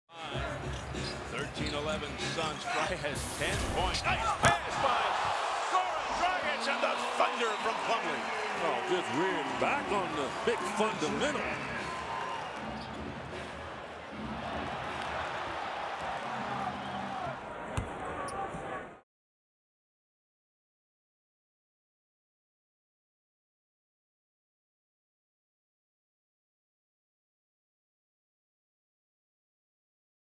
Miles Plumlee Throws Down the JAM Over Tim Duncan
Goran Dragic drives and dishes to Miles Plumlee, who uses the power stuff over Tim Duncan. for more highlights.